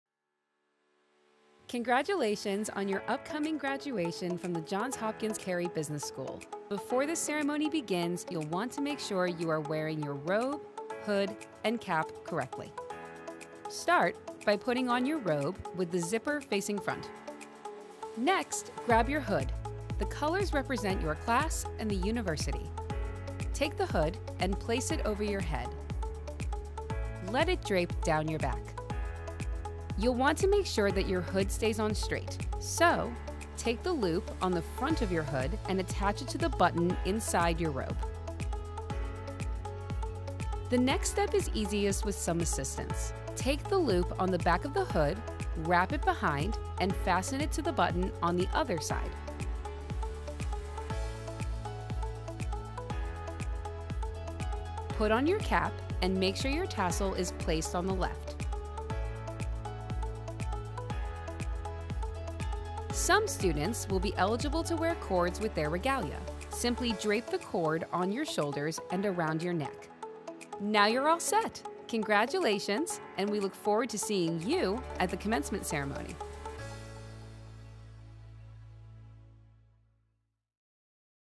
Johns Hopkins: How to wear your graduation regalia
Congratulations on your upcoming graduation from Johns Hopkins Carey Business School! Not sure how to wear your graduation regalia? Let us walk you through how to wear your gown, hood, and cap for the ceremony. We look forward to welcoming you to the Carey Business School alumni network!